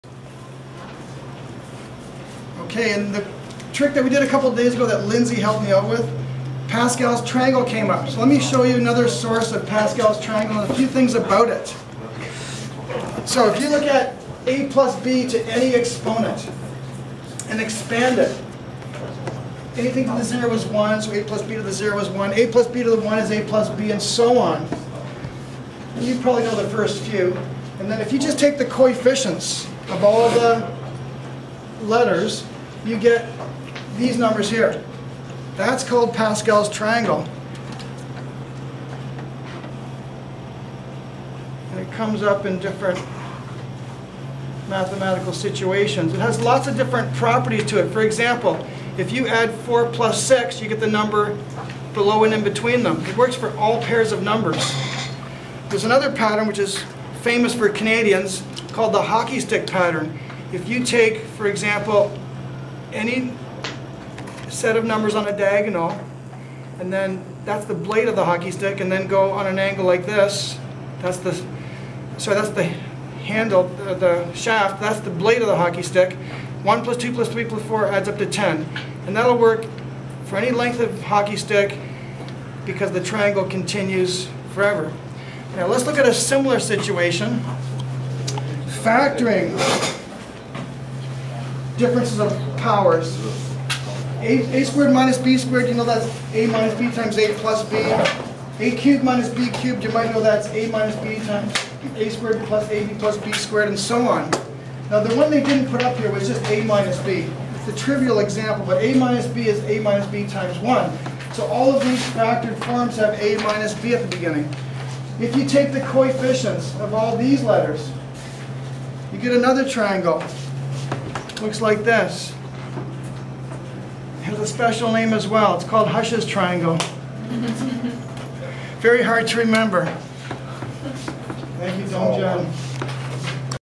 Pascal's triangle and Hush's triangle.
Mr. John Hush explains one source of Pascal's triangle (binomial powers) and a few of its properties. He then explains the source of Hush's triangle (factoring differences of powers) and its great simplicity!